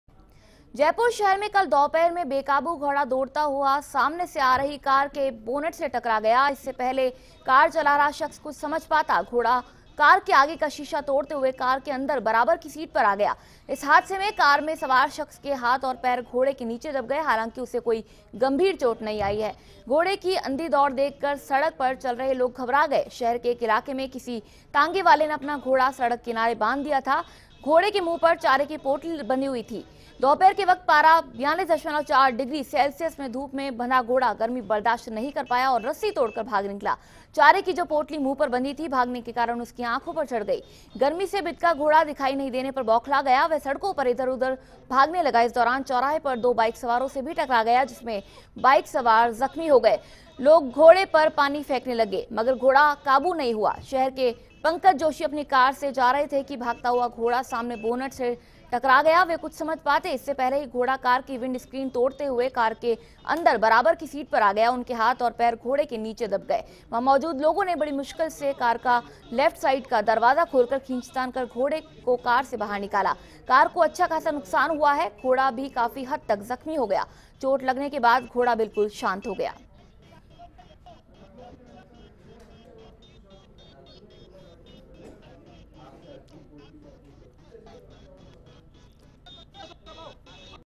जब चलती कार में अचानक सामने से घुस गया गर्मी से परेशान एक घोड़ा
एक बेकाबू घोड़ा दौड़ता हुआ सामने से आ रही कार के बोनट से टकरा गया। इससे पहले कि कार चालक कुछ समझ पाता, घोड़ा कार के आगे का शीशा तोड़ते हुए कार के अंदर बराबर की सीट पर आ गया। इस हादसे में कार में सवार शख्स के हाथ और पैर घोड़े के नीचे दब गए। हालांकि, गनीमत रही कि उसे कोई गंभीर चोट नहीं आई। वहीं घोड़े की अंधी दौड़ देखकर सड़क पर चल रहे लोग घबरा गए।